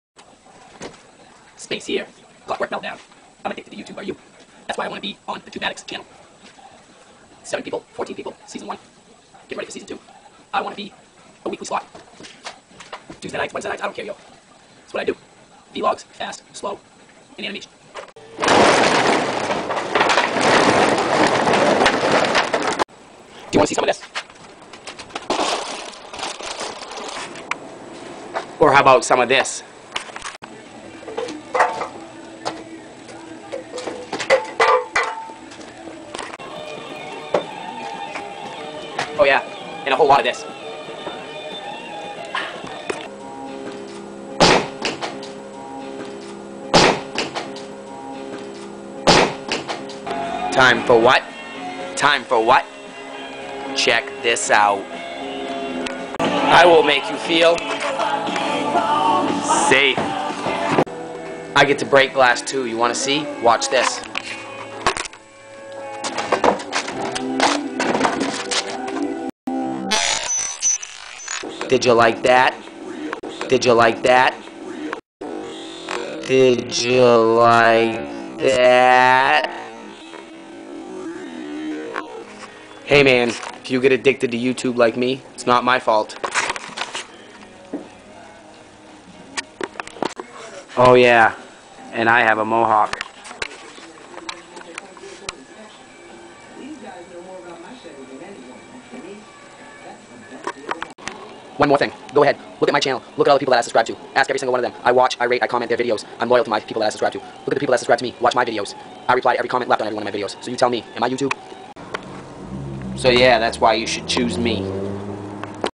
thetubeaddicts audition yo
just a short vid to show thetubeaddicts and their fans why i should be given a slot on their channel!!

rock on-space